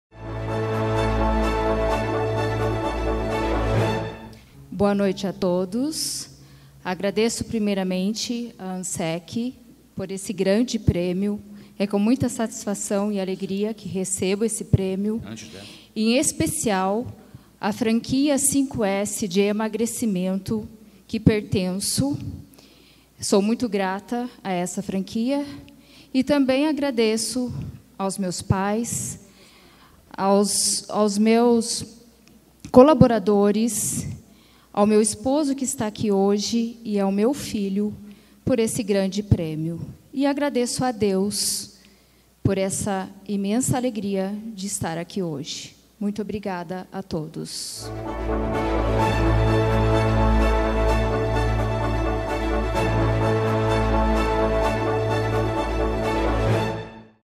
Emylene Struminski - Saúde Integrativa - Selo Referencia Nacional 2021
Empresa voltada à estética e saúde integrativa, estabelecida na cidade de
Taquarituba-SP há 12 anos, com filial no município de Itaporanga-SP, promove em seus pacientes os mais modernos, seguros e eficazes procedimentos estéticos em geral; destacando-se ainda por seus serviços de franquia e licenciamento de métodos de emagrecimento como o “5S” e o “Decidi Emagrecer”, cujo nohall lhe garantiu inúmeras vitórias no combate aos diversos graus de obesidade.
Reestabelecendo saúde, qualidade de vida e, sobretudo, promovendo uma máxima auto estima e plena felicidade aos seus pacientes.